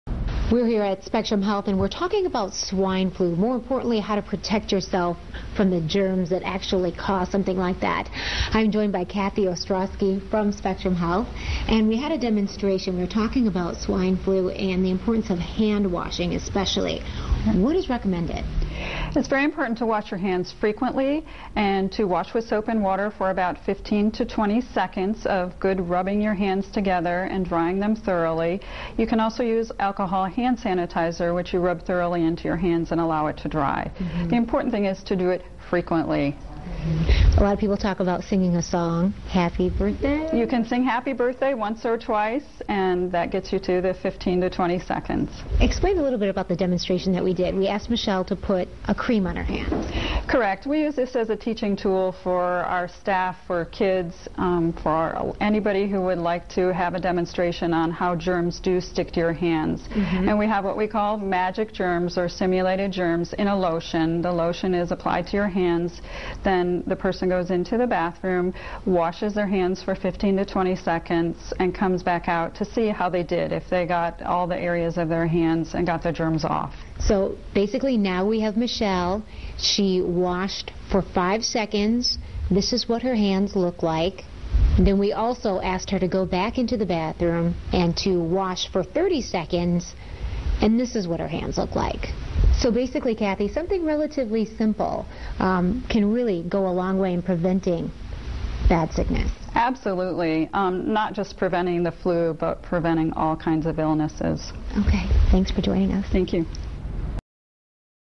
Importance of hand-washing
Cathy Ostrowski of Spectrum Health demonstrates the best way to protect yourself from swine flu and the germs that can cause the illness.